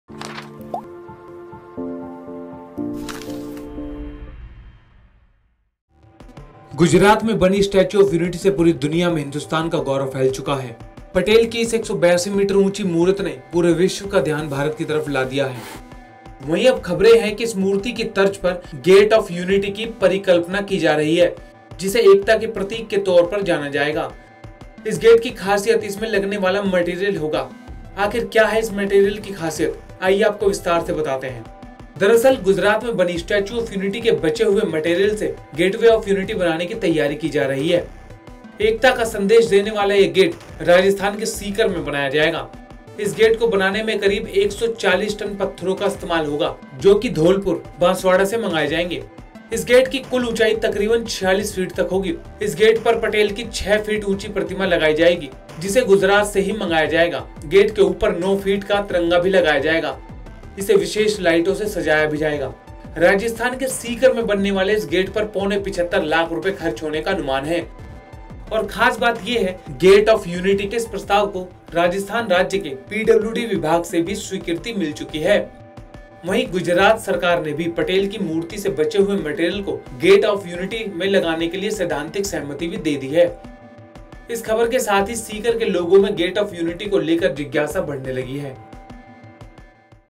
Statue of Unity के बाद अब Rajsthan में बनेगा Gate of Unity | Khabar Update
स्टेच्यू ऑफ यूनिटी के बाद अब गेट ऑफ यूनिटी बनेगा. आखिर कैसा और किन खासियतों से लैस होगा यह गेट? देखिये हमारी खास रिपोर्ट-